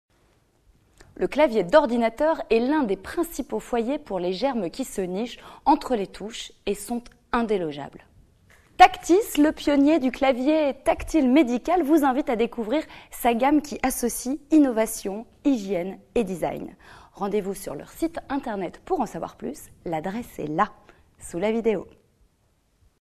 Claviers médicaux
Les claviers médicaux Tactys sont des outils efficaces dans la lutte contre les contaminations manu-portées.

Pionner des claviers tactiles médicaux, Tactys édite une gamme complète qui associe innovation, hygiène et design.
La surface en verre trempé se désinfecte d’un geste, assurant ainsi gain de temps et qualité d’asepsie. La technologie par effet capacitif permet une frappe sans appui, confortable et rapide.

Tous les claviers médicaux Tactyls sont :
- Etanches et lisses en verre trempé
- Inaltérables toutes faces
- Tolérants à tous les agents de désinfection
- Compatibles avec des gants même doublés
- 100% compatibles PC/MAC et Plug and play

Claviers médicaux tactiles, étanches, lisses et inaltérables en verre trempé. Chez Tactys, l’innovation et le design sont au service de l’hygiène…

"Aidez Sofian dans son combat contre l'ataxie telangiectasie"